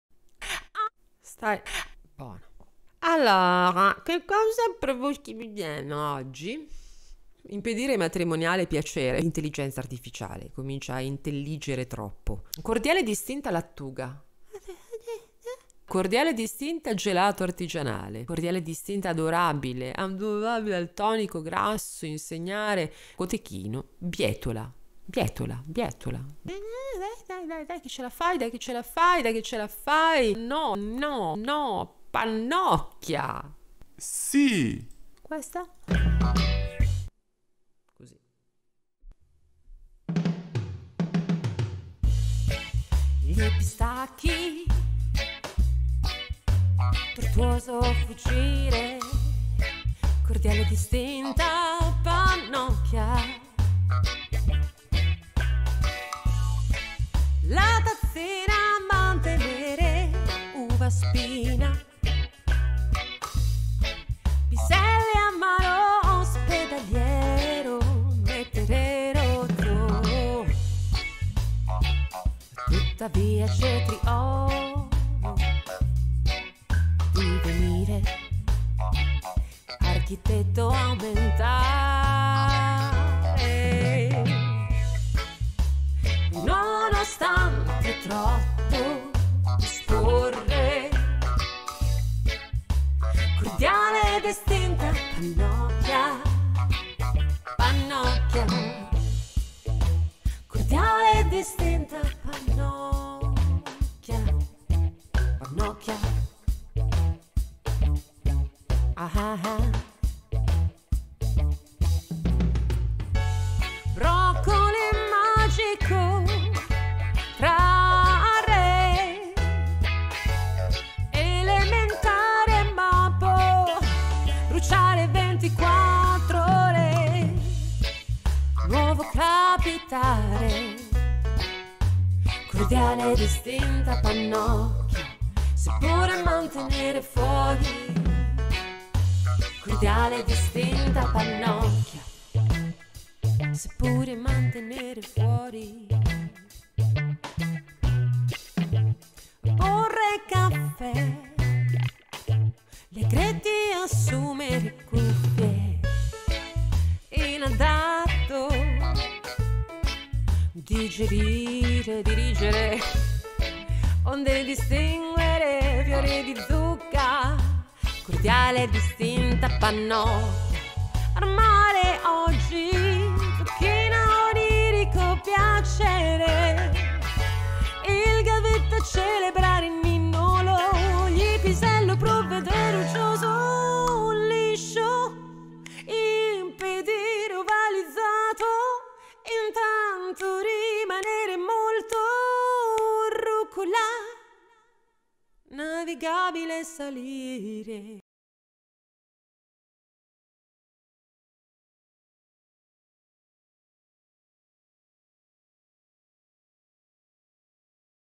Una pannocchia anche per te
Pannocchia: Infiorescenza composta indefinita.
Decisamente distinta e pare anche cordiale la pannocchia.
Sulla griglia in un pomeriggio estivo non può che essere celebrata a ritmo di reggae 🌽

Se ti è venuta voglia di mais
❗️Condividi

Se vuoi duettare con me